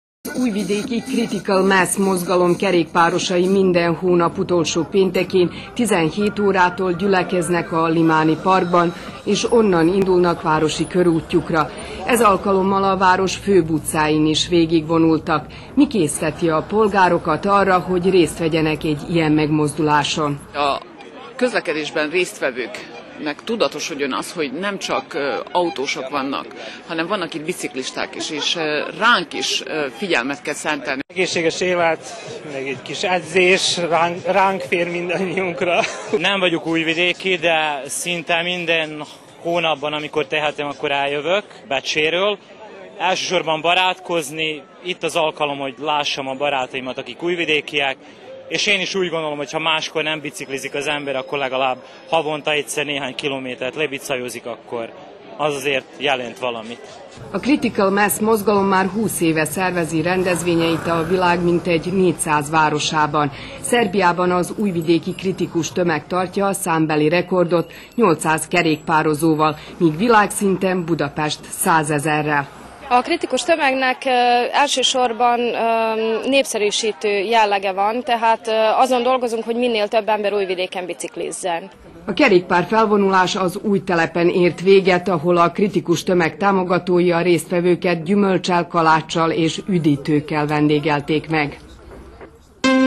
Három éves a Kritikus Tömeg
Az újvidéki Kritikus Tömeg megünnepelte megalakulásának harmadik évforulóját. Ez volt eddig a legnépesebb kerékpáros felvonulás a székvárosban.